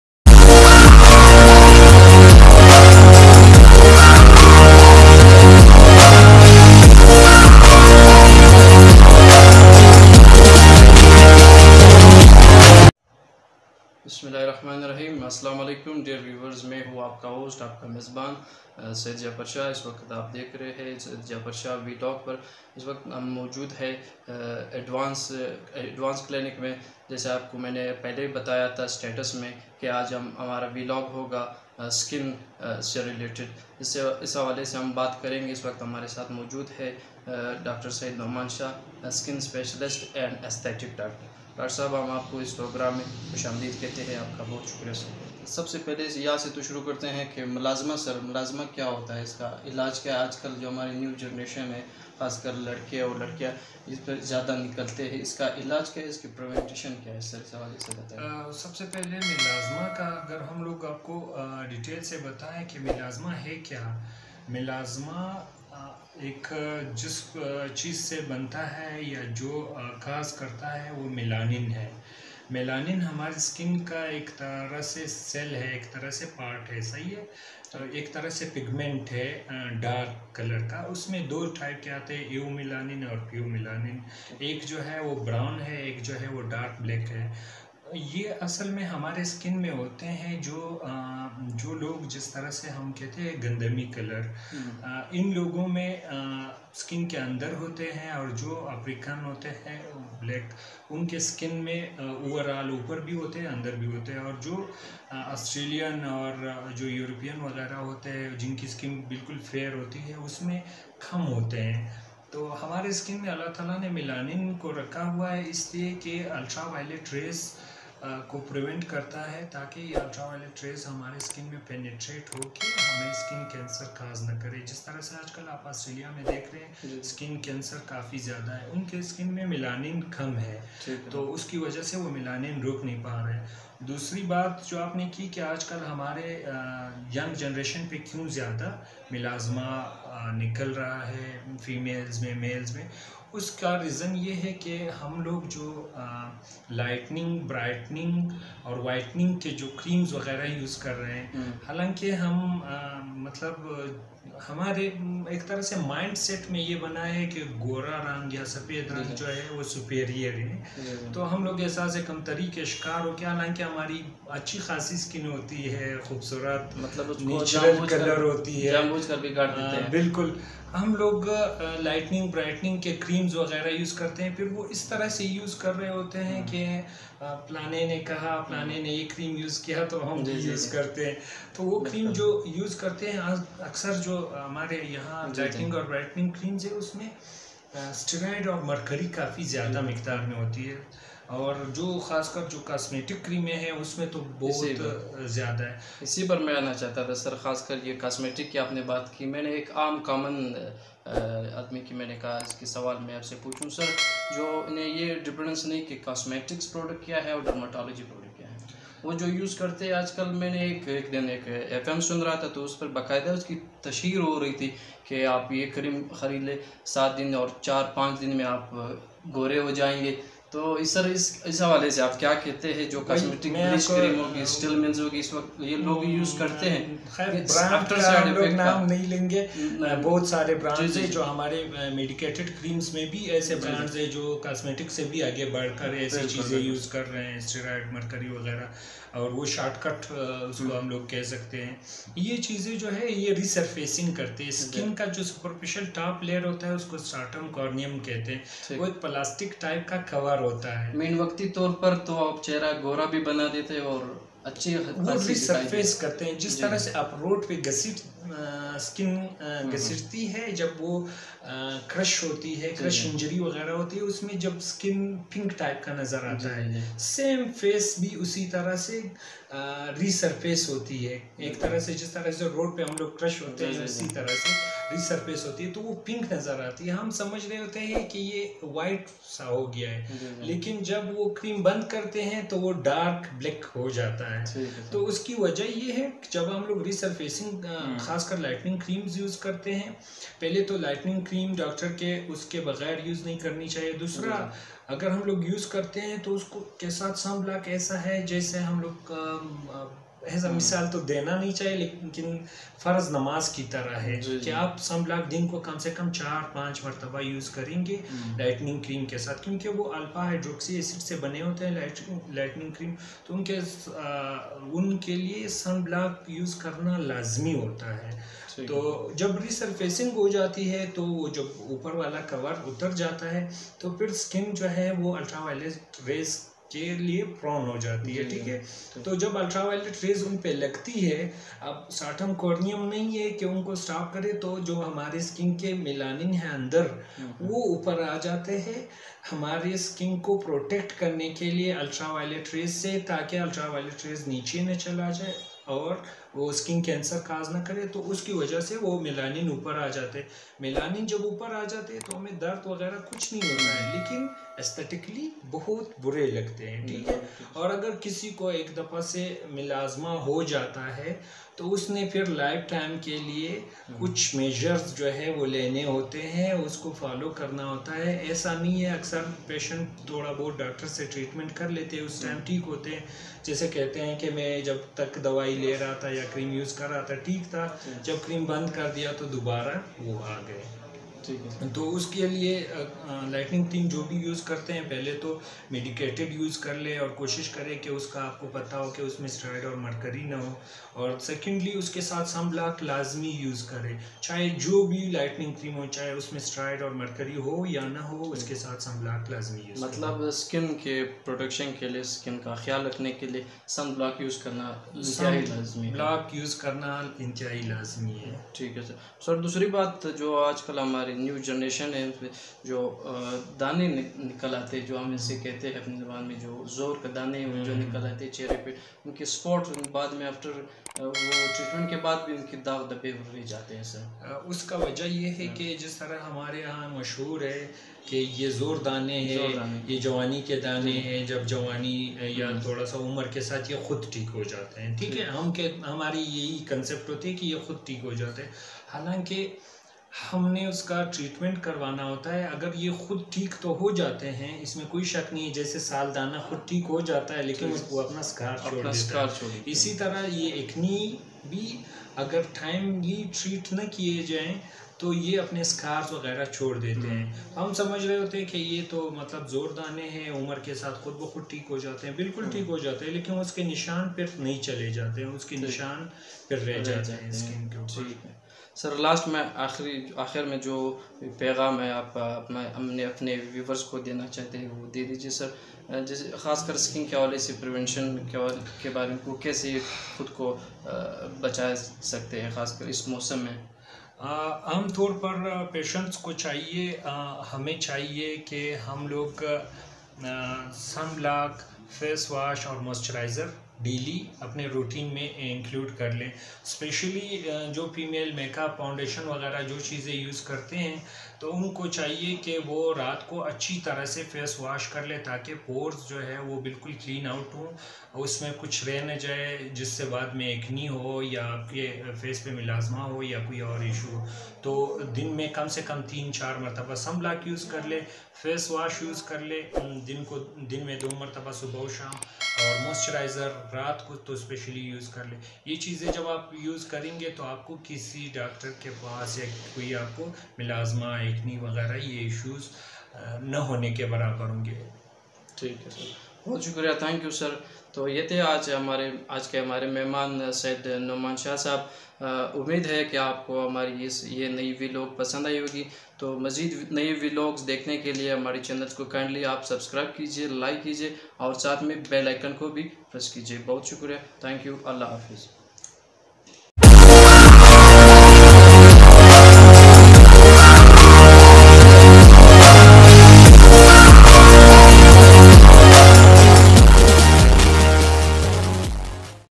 With Dr Syed Noman Shah Cosmetic Aesthetic & Regenerative Skin Specialist PART(1) PART(2)Coming soon
Advance Cosmetic skin Clinic Quetta bring you complete range of Cosmetic Aesthetic & Regenerative Skin treatments